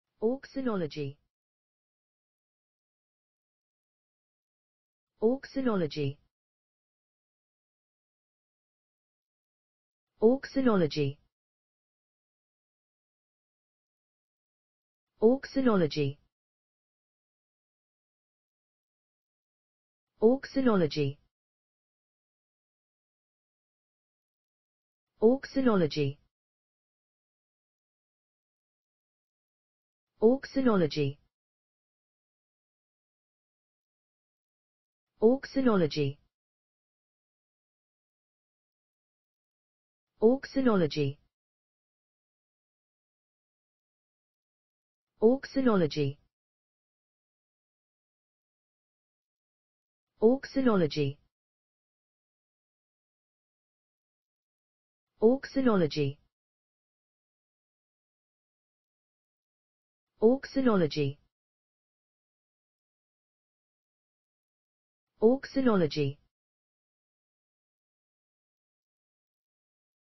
auxanology: How to pronounce auxanology in english (correct!).Start with A. Learn from me.
Follow me!Learn from me how to pronounce the word auxanology in english.This video shows you how to pronounce the word auxanology in english.

auxanology